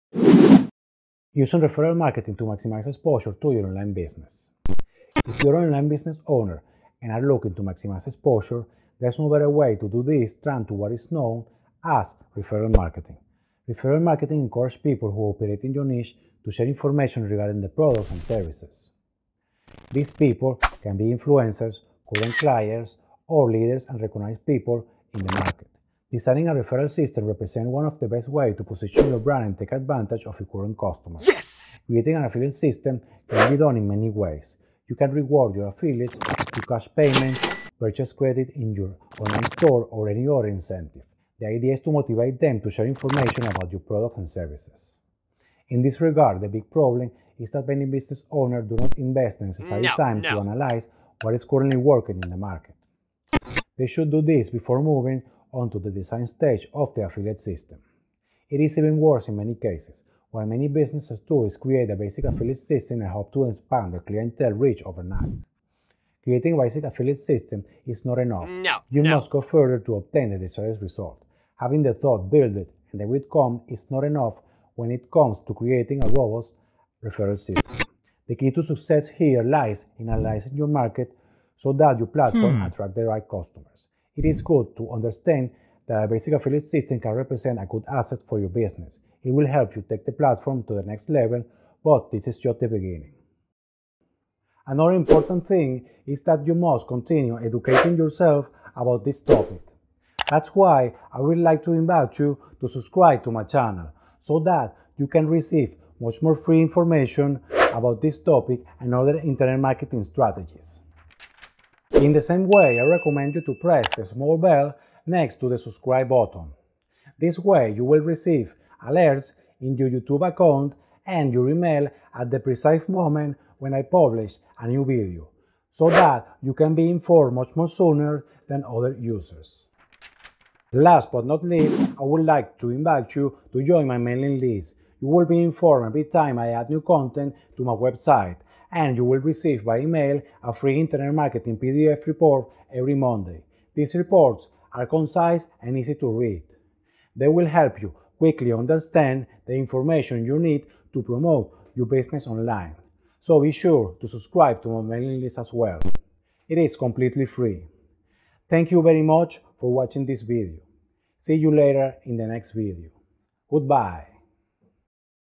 Using Referral Marketing To Maximize Exposure To Your Online Business
In this video, I'm going to explain to you: Using referral marketing to maximize exposure to your online business.

If you are an online business owner and are looking to maximize exposure, there is no better way to do this than through what is known as referral marketing. Referral marketing encourages people who operate in your niche to share information regarding their products and services. These people can be influencers, current clients, or leaders and recognized people in the market.

Designing a referral system represents one of the best ways to position your brand and take advantage of your current customers. The idea is to motivate them to share information about your products and services.

In this regard, the big problem is that many business owners do not invest the necessary time to analyze what is currently working in the market. Having the thought: "build it, and they will come" is not enough when it comes to creating a robust referral system. The key to success here lies in analyzing your market so that your platform attracts the right customers. It will help you take your platform to the next level, but this is just the beginning.

Ramon Tarruella